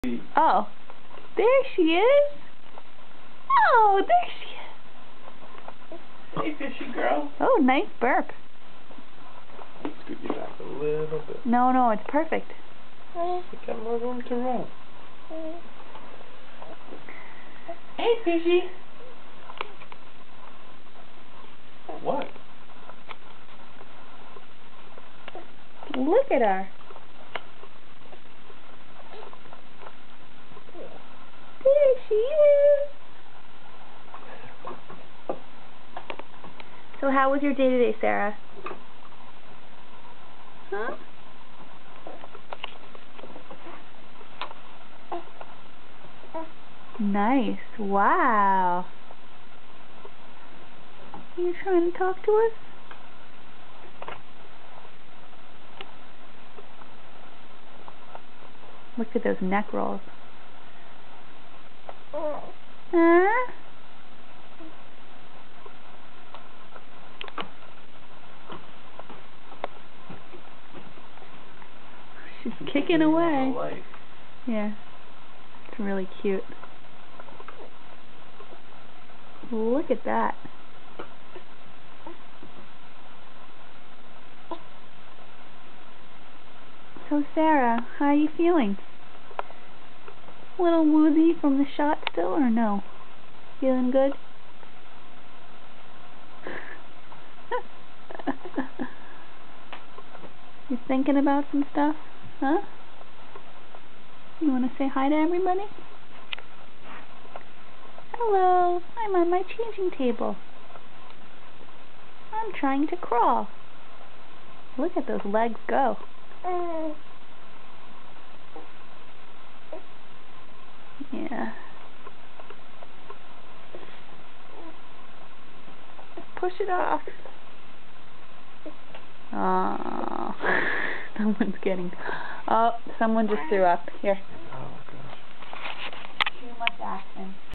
Sarah Lifting Her Head on the Changer
Sarah lifting her head in the changer